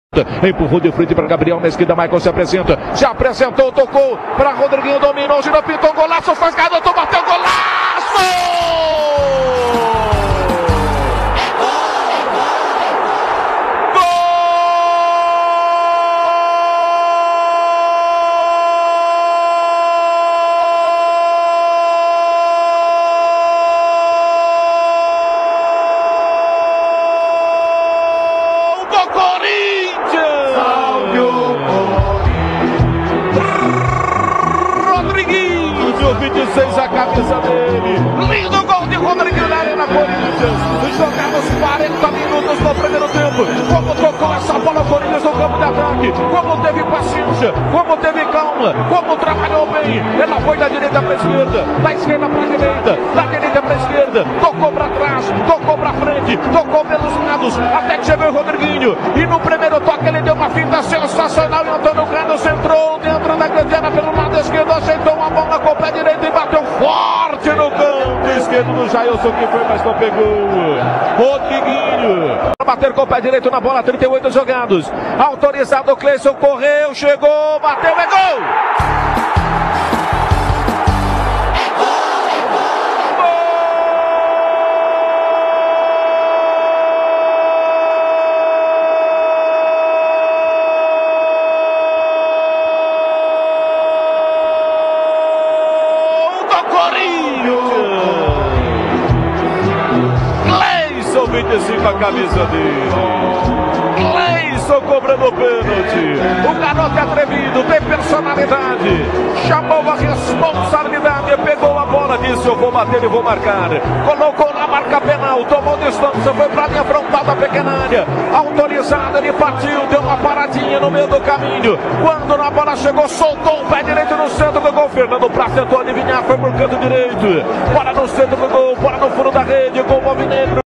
24/02/18 | Corinthians 2 x 0 Palmeiras - Paulistão 2018 | Nar. Hugo Botelho/Rádio Capital
9° rodada do Paulistão 2018
SC Corinthians Paulista 2 x 0 SE Palmeiras
Data: 24/02/18
⚽ Rodriguinho e Clayson .
Estádio: Arena Corinthians
Narração: Hugo Botelho
Rádio: Capital